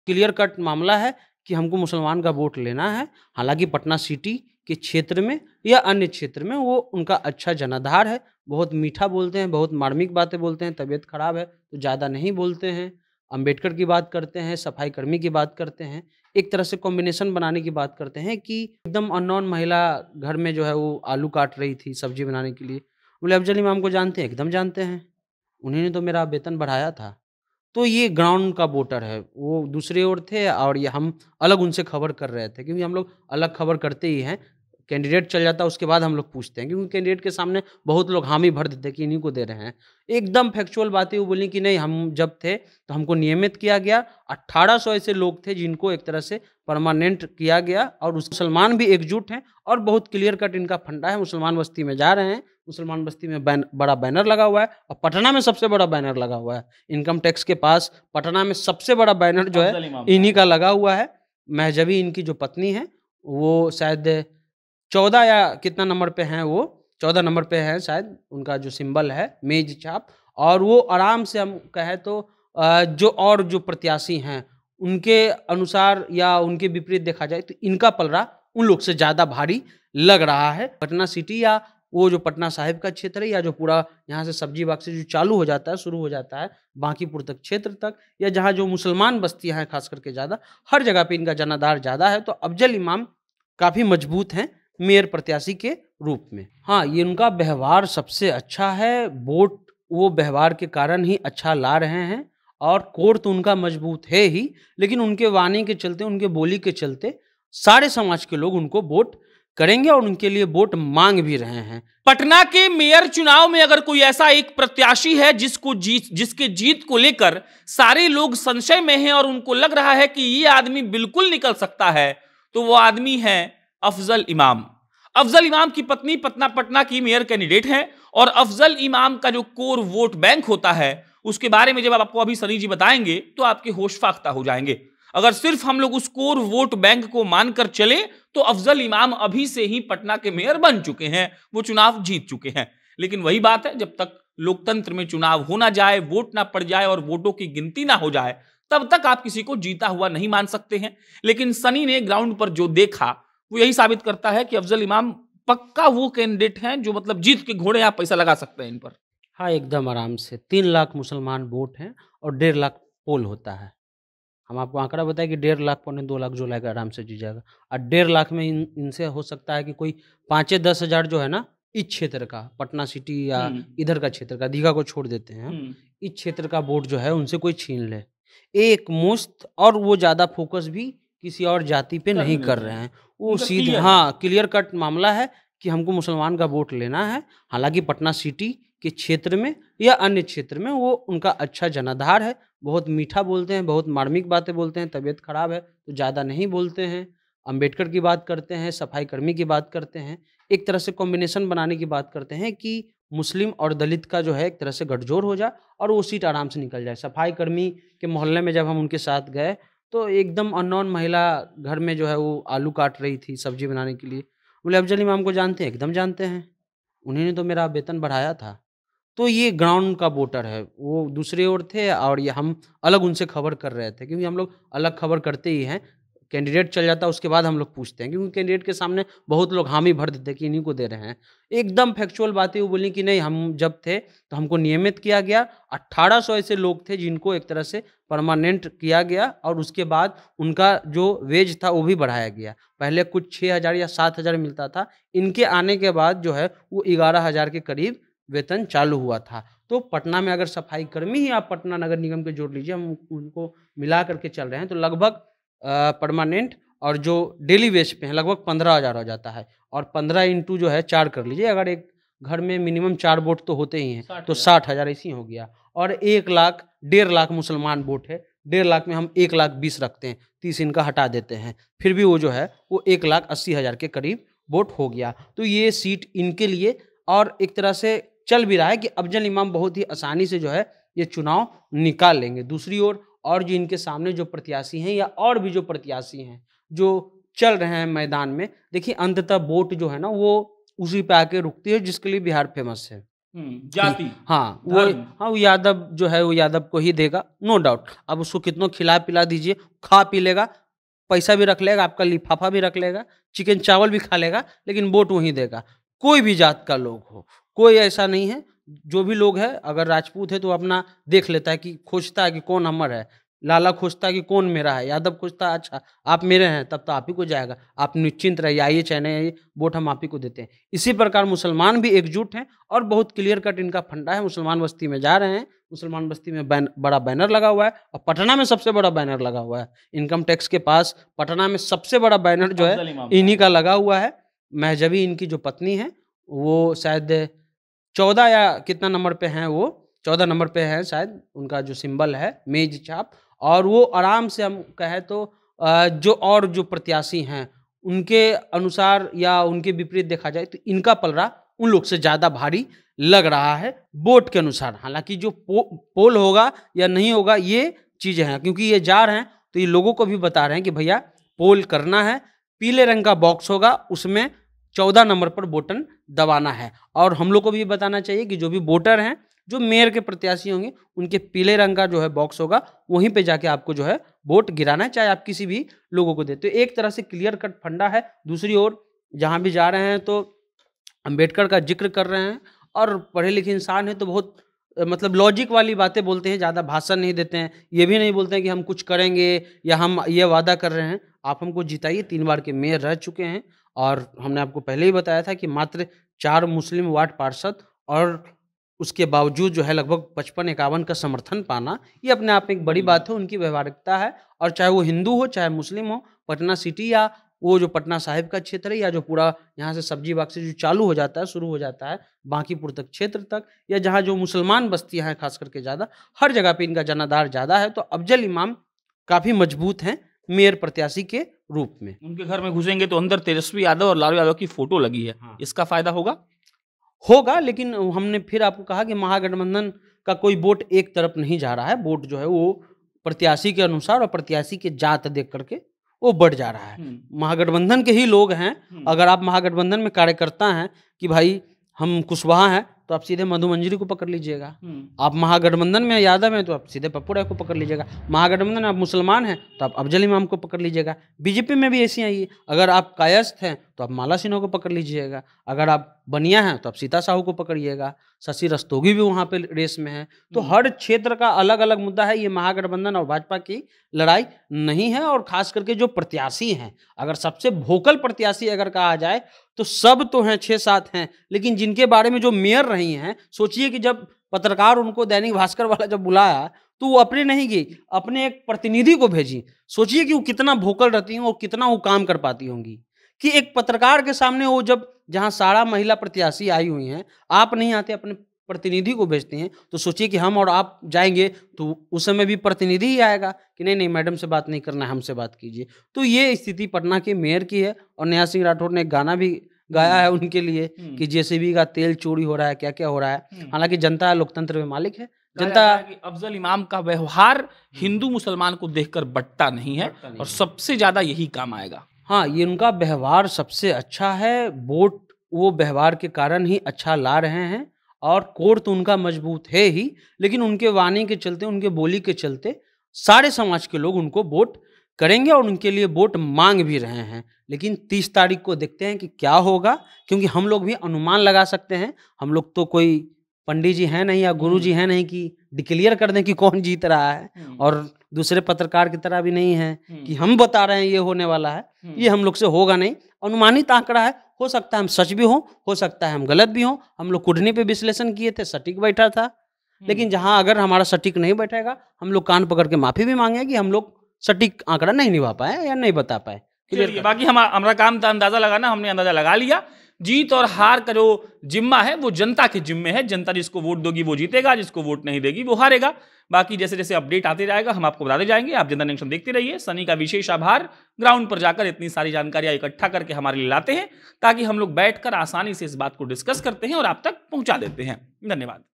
Patna के सारे मुसलमान Afzal Imam को मेयर बनाकर ही मानेंगे, Tejashwi का भी साथ मिलेगा!
तेजस्वी और भाजपा की बात नही है, पटना के मुसलमान अफजल इमाम को जिताकर मेयर बना देंगे! समझ लीजिए कि कैसे अफजल इमाम खेल करेंगे और मेयर बन जाएंगे!